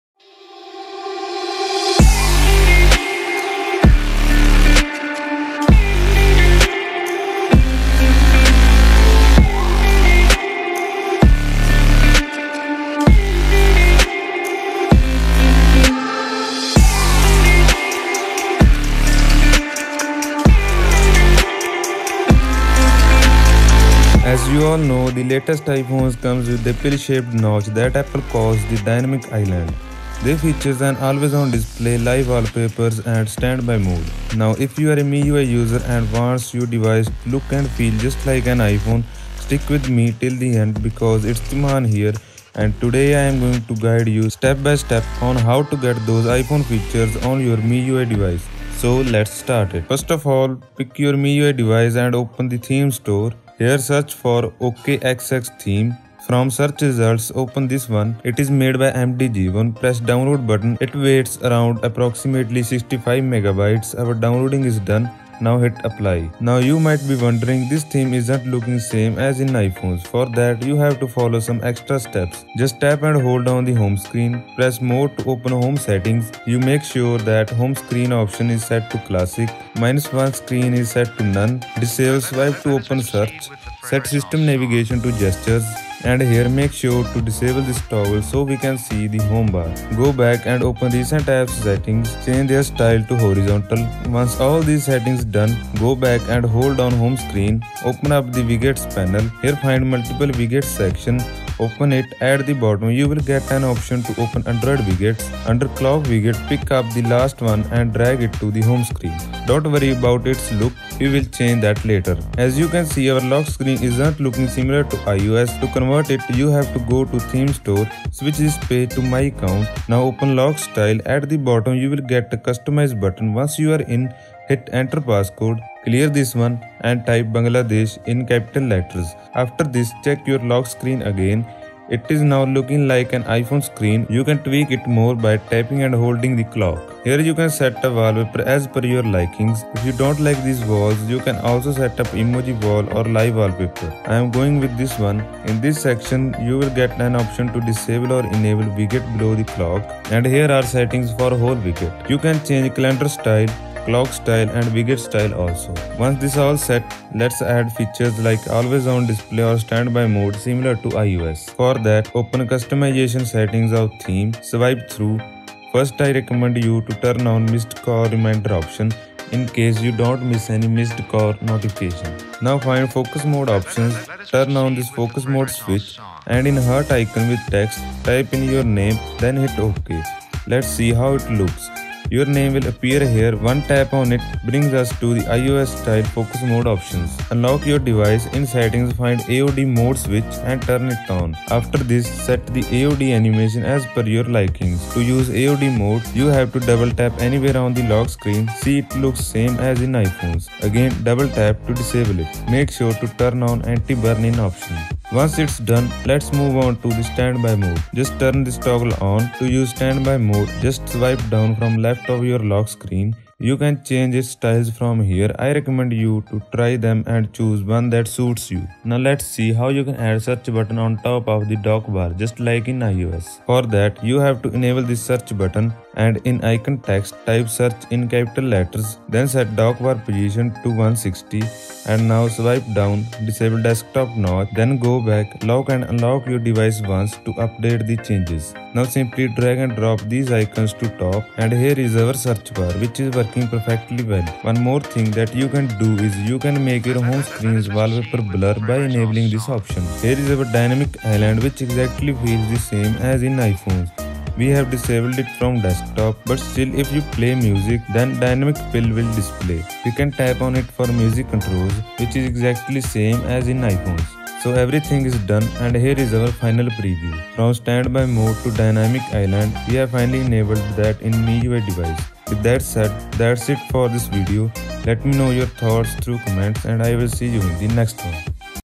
Best iOS Theme For MiUi (2024)
🎬 Content Guide
00:00 Introduction
00:50 Installing the Theme
01:06 MiUi to iOS Home Settings
01:33 iOS Home Widget
01:52 MiUi to iOS Lockscreen Settings
02:41 Advanced Theme Settings - Missed Call
02:52 Enabling iOS Focus Mode
03:07 Enabling iOS AOD Mode
03:52 Enabling iOS StandBy Mode
04:10 Enabling iOS Search Button
iOS Dynamic Island in MiUi
04:24 The End